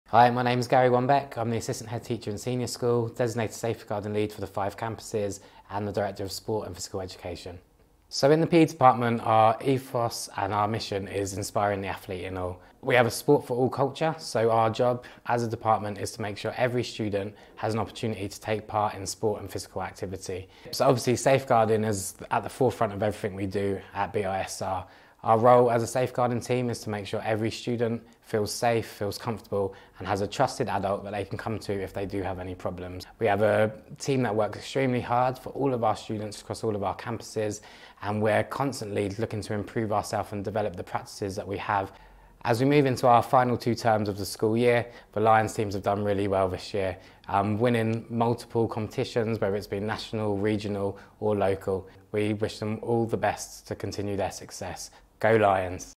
Meet our Safeguarding Lead across our 5 campuses - Mr Wambeek
Mr Wambeek has 17 years of teaching experience across the UK, Africa, Asia and the Middle East. He is designated Safeguarding Lead across our five campuses, a valued member of our SLT in his role as Assistant Headteacher, Director of Sport and Physical Education at our AlHamra campus as well as playing a part in our well-being pillar. ❤️